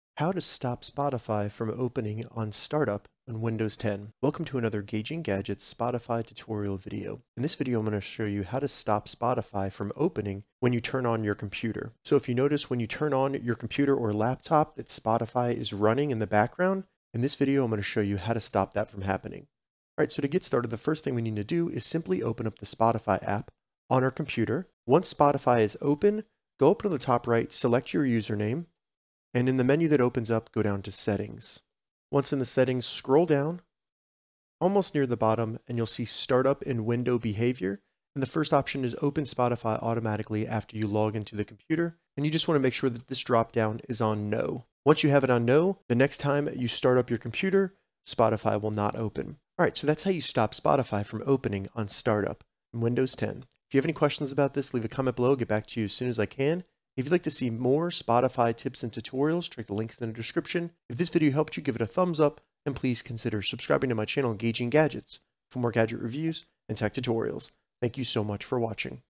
How to Stop Spotify from Opening on Startup - 2022
How to stop Spotify from opening on startup? In this tutorial, I show you how to stop Spotify from opening on startup. Turning off Spotify on start up will stop Spotify from automatically opening when you turn on your computer. Having too many programs open on startup can slow down your computer.

Follow the steps below to stop Spotify from opening on Startup:
1. Open Spotify on your Windows laptop or computer.
2. Select your username in the top right of the app.
3. Select Settings.
4. Scroll down in the Spotify settings and find Open Spotify automatically after you log into the computer. Ensure No is selected in the dropdown.